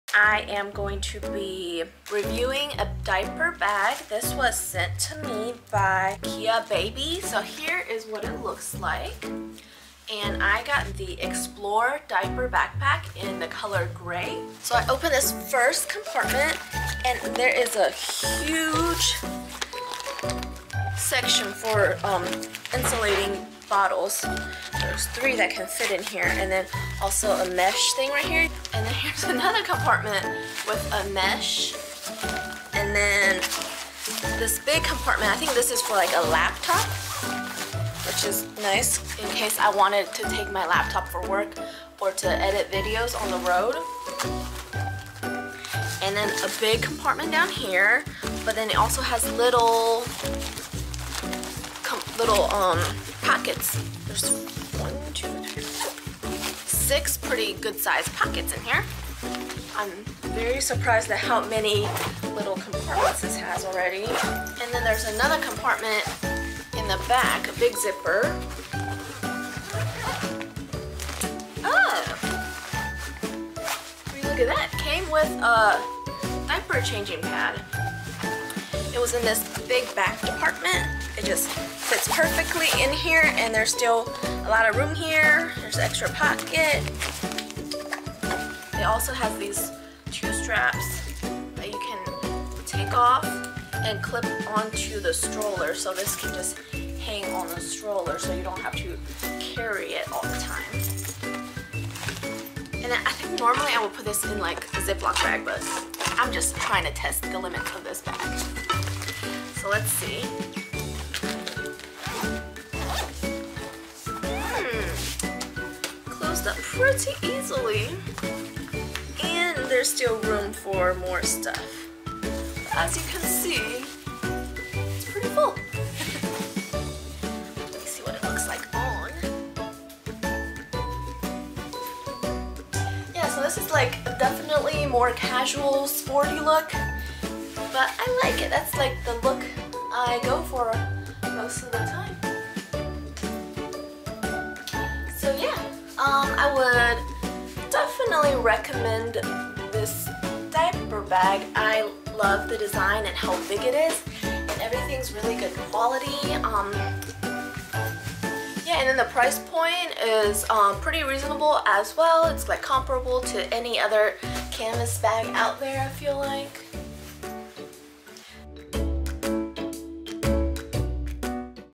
A New Adventure Awaits with Explorer Diaper Bag | Stylish Backpack Design!
Our Explorer Diaper Backpack is perfect for Adventure with your baby!👶

Specially-designed with functionality and style for all parents to enjoy parenthood without the frustrations of digging around inside your nappy bag. This is the best choice for moms & dads when going outdoors with your baby.👨‍👩‍👧‍👦💞

𝑨𝒎𝒂𝒛𝒐𝒏 𝑳𝒊𝒏𝒌